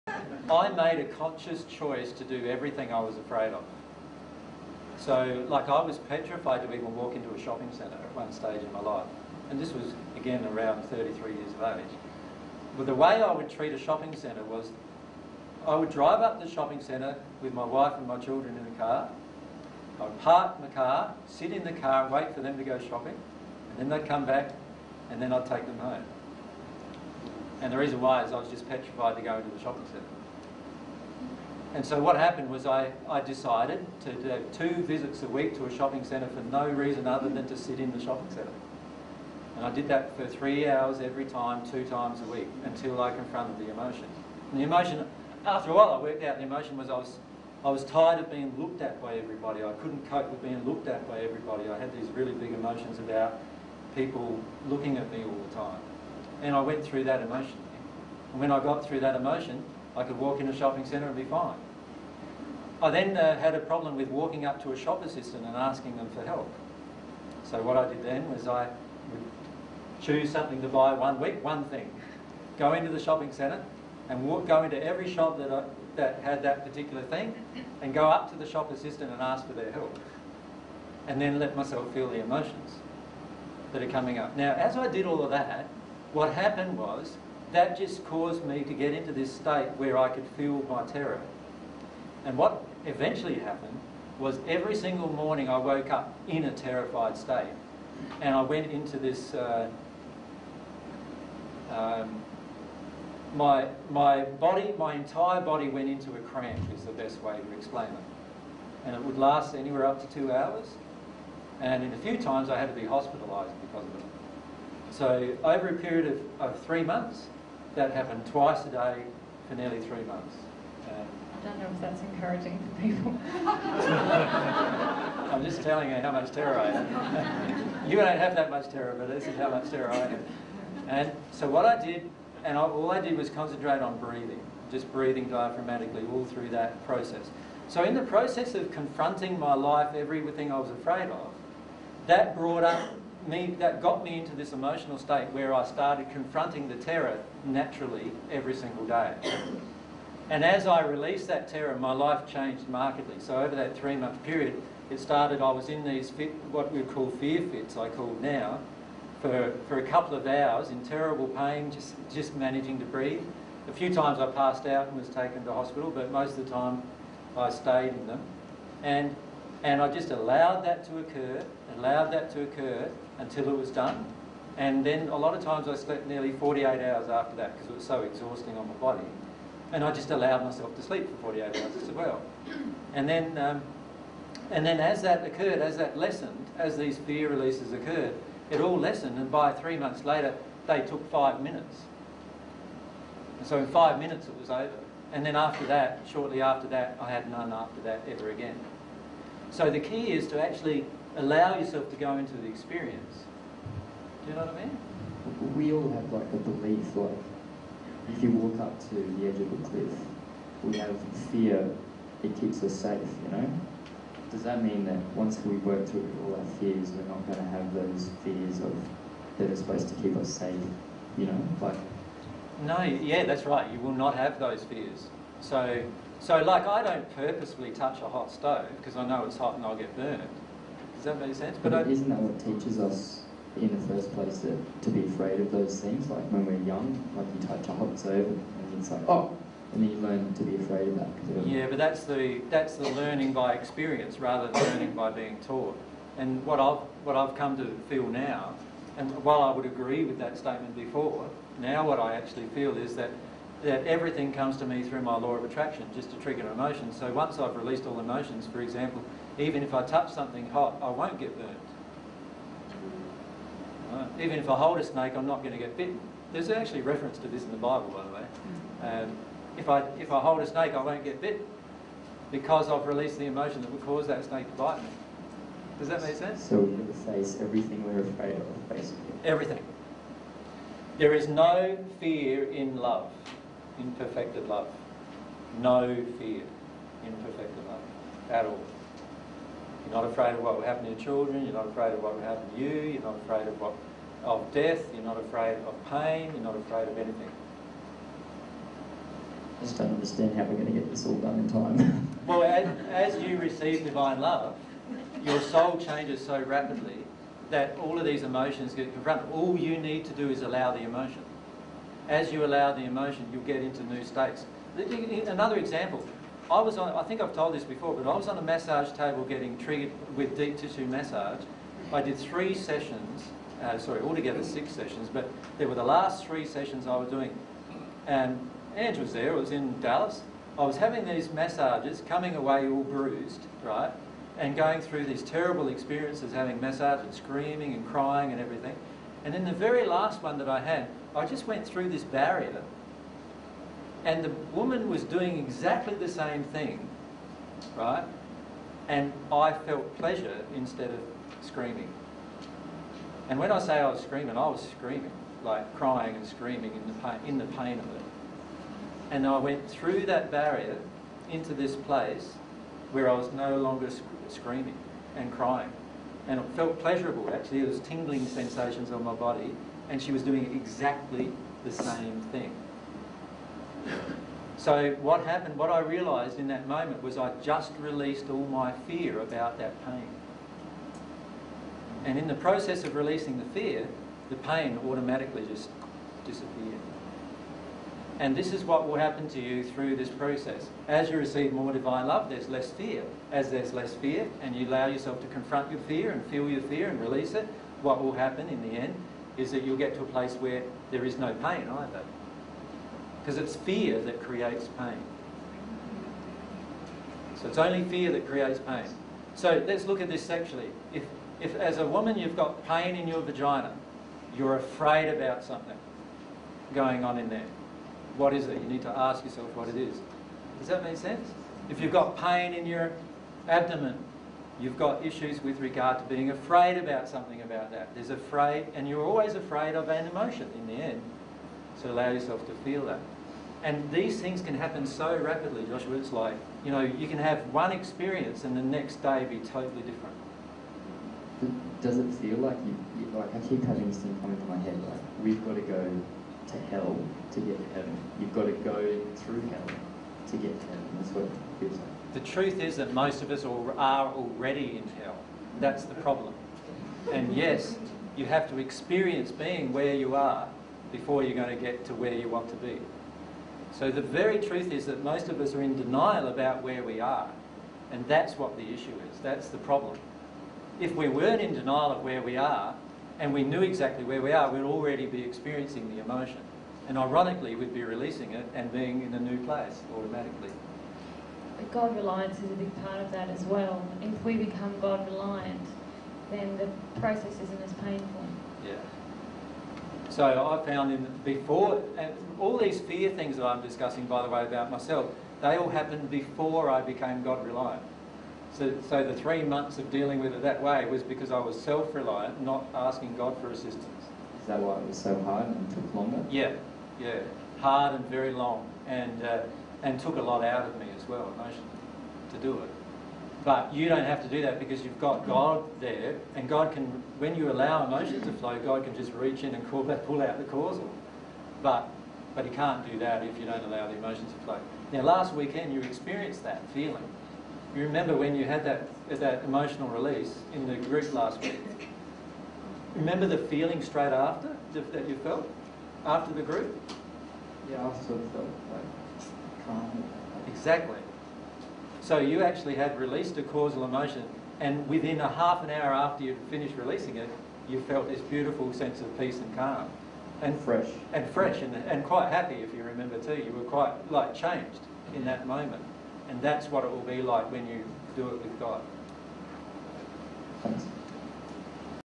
Jesus & Mary on Working Through Fear and Terror
Jesus and Mary discuss working through and releasing fear and terror.

This is a section of the presentation by Jesus and Mary entitled "20090315 Human Relationships - Sex & Sexuality S2P2".

dtstart:004413:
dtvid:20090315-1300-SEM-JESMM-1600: